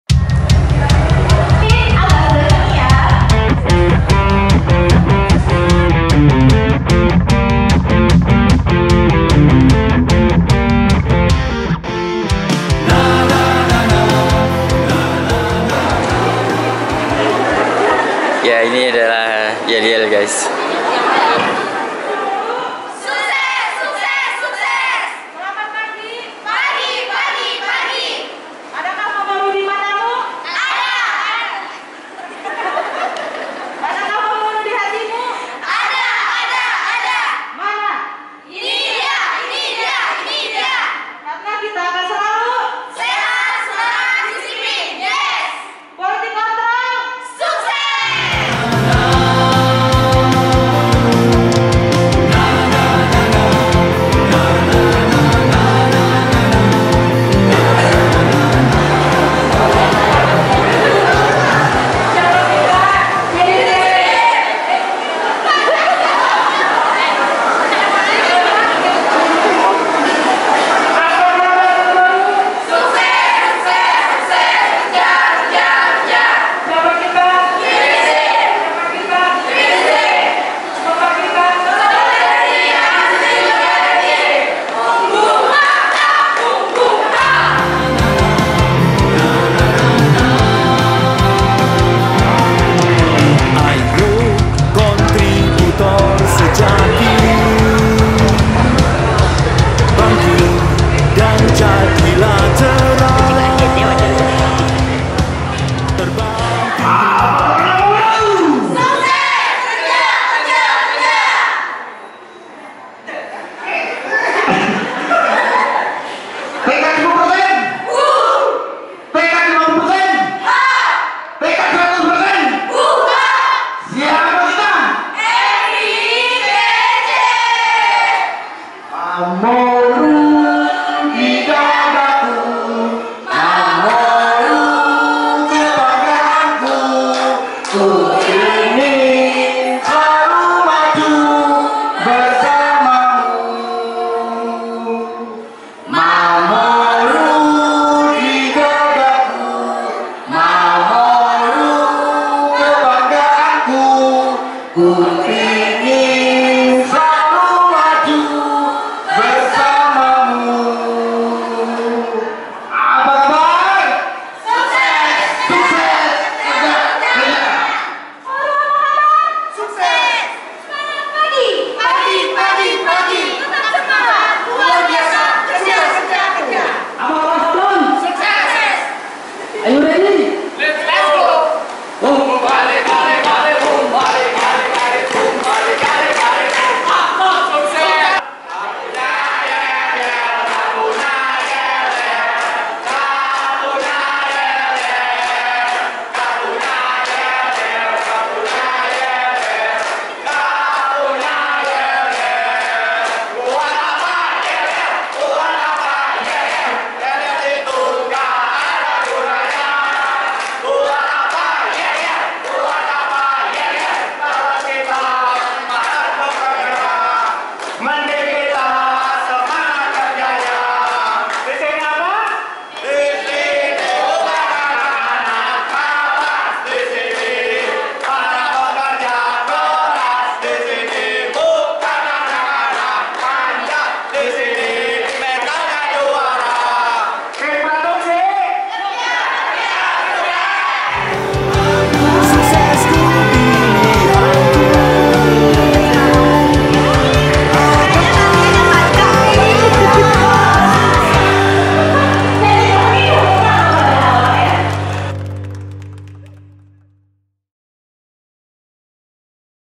KOMPETISI YEL-YEL TIAP DEPARTEMEN
salah satu cara membentuk budaya positif di perusahaan adalah dengan membuat yel-yel